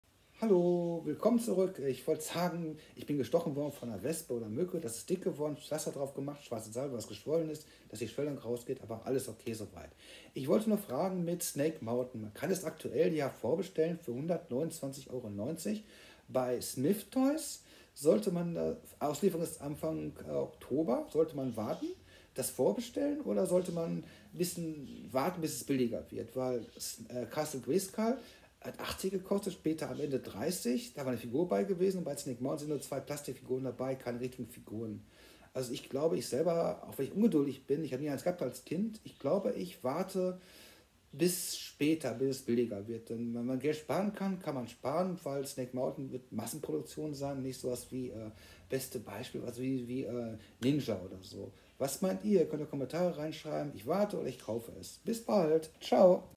173 - Snake Mountain Origins - Jetzt Vorbestellen oder besser warten ???
Aktuell Snake Mountain für 129,90 Euro bei Smyths Toys zum vorbestellen